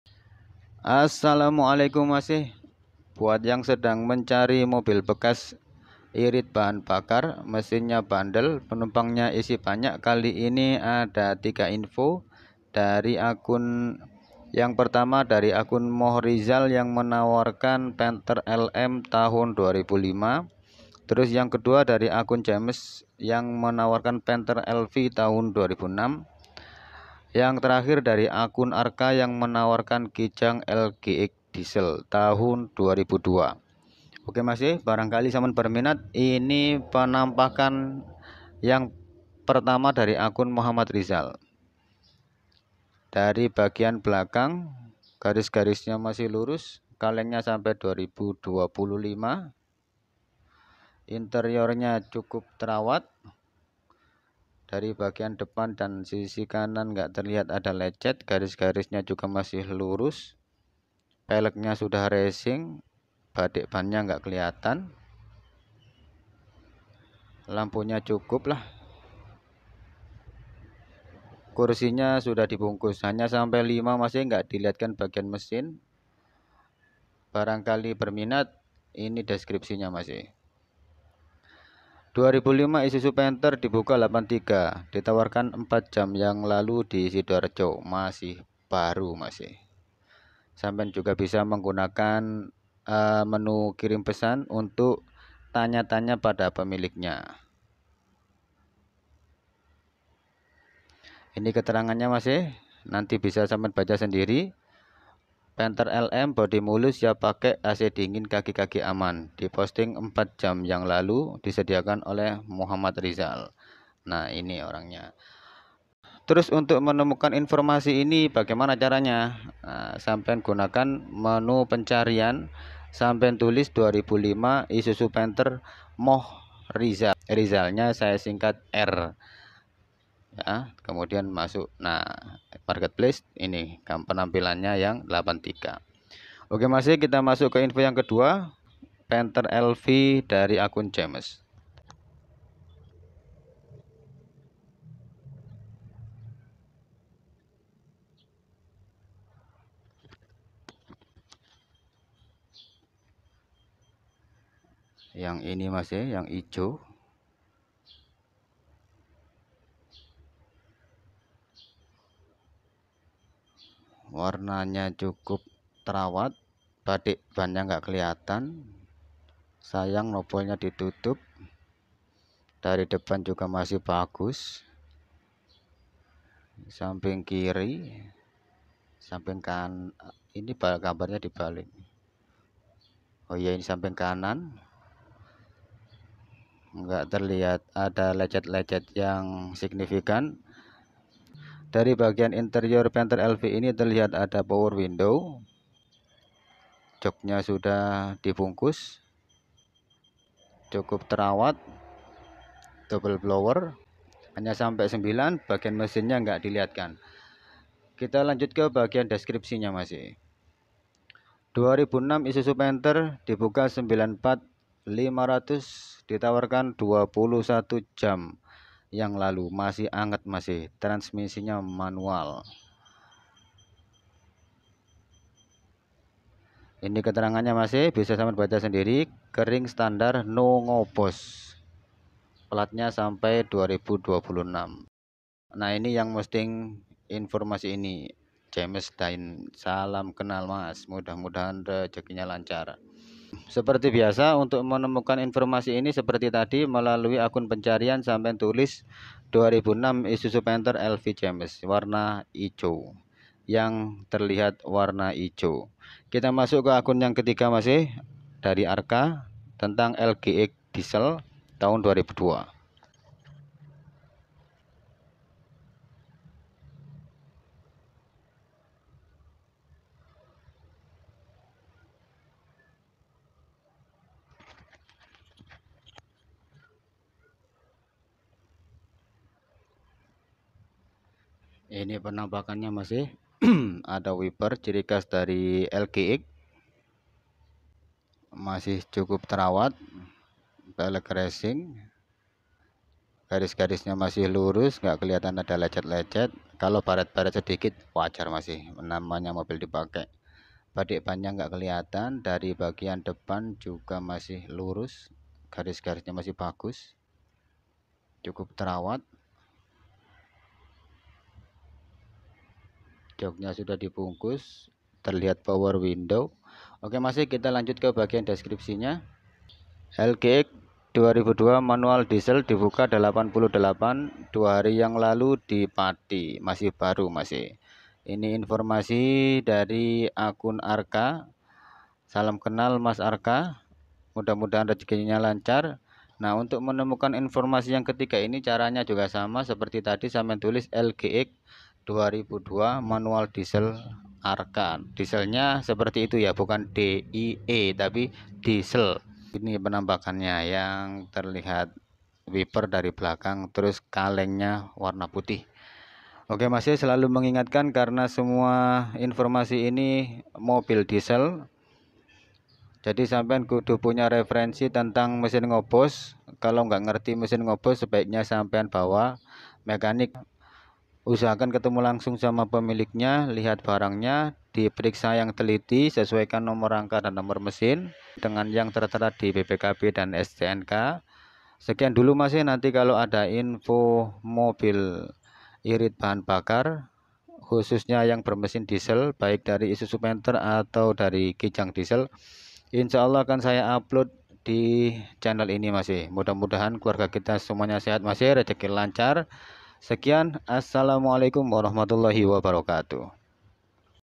Berburu Mobil Impian - Kijang LGX Diesel dan Isuzu Panther Murah
buat yang sedang mencari mobil bekas irit bahan bakar harga relatif murah mesin bandel isi penumpangnya banyak. kali ini ada info:
kijang lgx diesel solar
isuzu panther lm
isuzu panther lv
barangkali berminat dan dekat dengan tempat tinggal sampean, silahkan disimak. informasi ada dalam video.
jika tidak bisa menemukan informasi ini, bisa jadi mobilnya sudah laku.

kijang lgx diesel solar murah, panther lm murah, panther lv murah